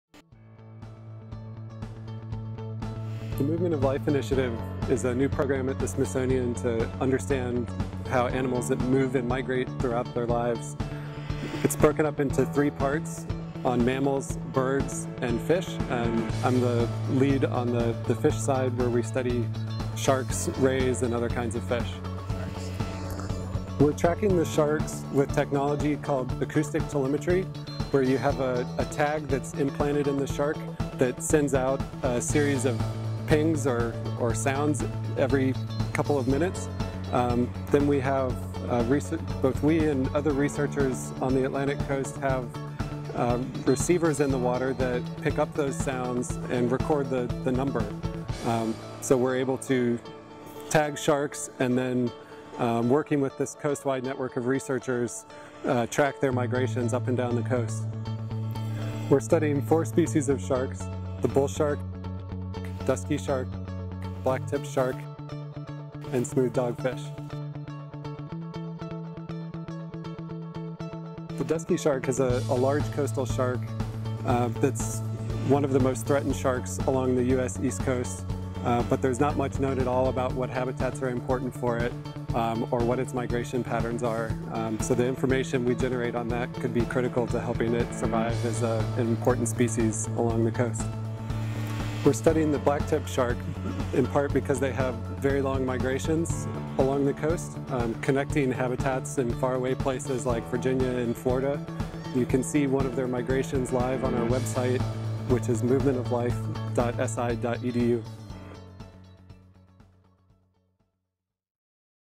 Shark Tagging: Smithsonian Movement of Life Initiative
The Movement of Life Initiative is a developing program in animal tracking research conducted by Smithsonian Institution researchers and their colleagues. As part of the program, scientists Matt Ogburn and Charles Bangley at the Smithsonian Environmental Research Center are tracking sharks along the East coast of the United States. They’re focusing on four species of sharks: Bull, Blacktip, Dusky, and Smooth Dogfish. These four sharks are regulars in the Chesapeake Bay region and fulfill a variety of ecological roles, but much basic information about their movement ecology within the estuary and beyond is still unknown. Ogburn and Bangley are tracking the sharks using a system called “acoustic telemetry,” equipping the animals with tags that emit a series of pings, which receivers along the Atlantic can pick up. Through acoustic telemetry, the scientists aim to better understand habitat use and migration behavior among these coastal sharks in the U.S. Mid-Atlantic region.

Thumbnail Photo: Smooth Dogfish shark by Chuck Bangley